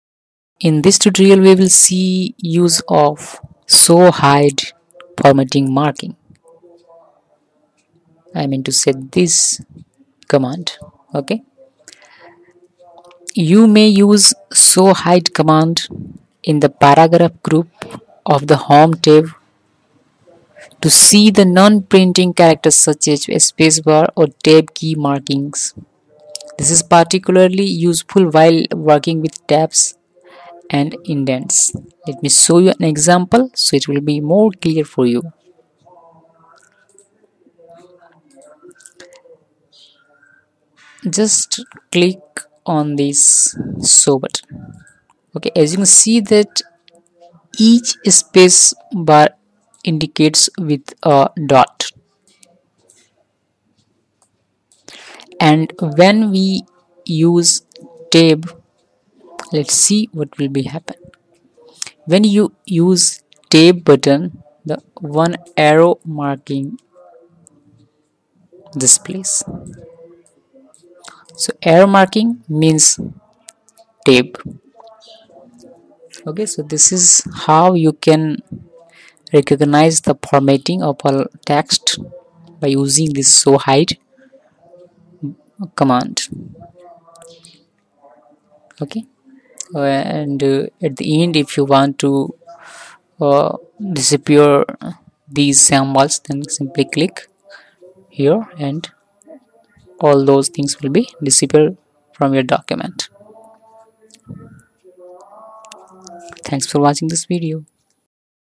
Microsoft Office Word 2010 Show or Hide Format Mark
Microsoft Office word 2010/2007/2013
If you are using Word 2007 then you have to click 'Microsoft Office' button instead of 'File' and the remain process are same as word 2010/2013.
In this tutorial we will learn how to Show or Hide Format Mark.